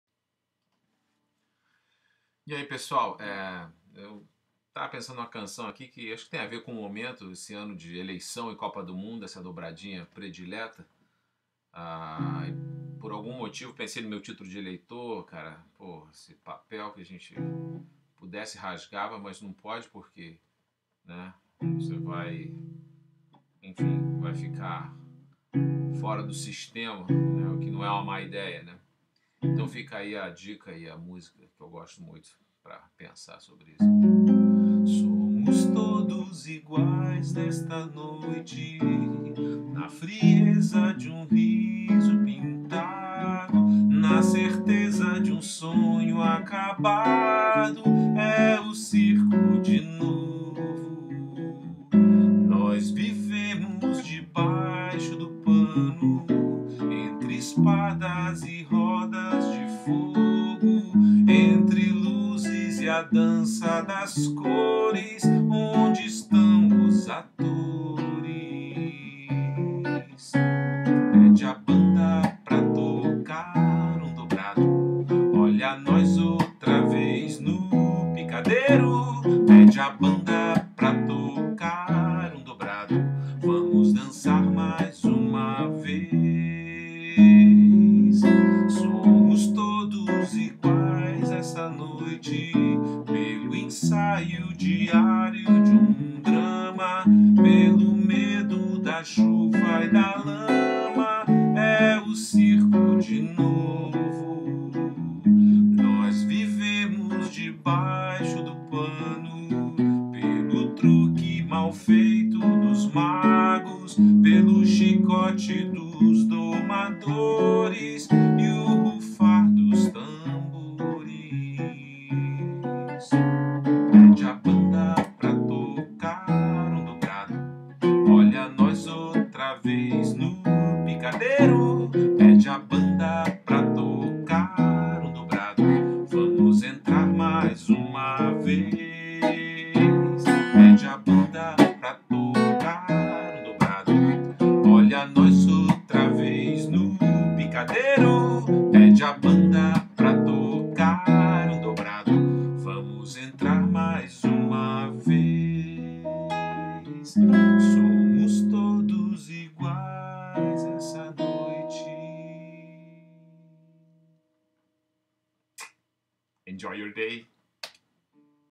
Canção do dia 1
Música de um grande compositor brasileiro, para refletir sobre o momento, mais específicamente sobre a dobradinha (Copa do mundo/Eleições). Deixe seu recado, subscreva meu canal, enfim, participe!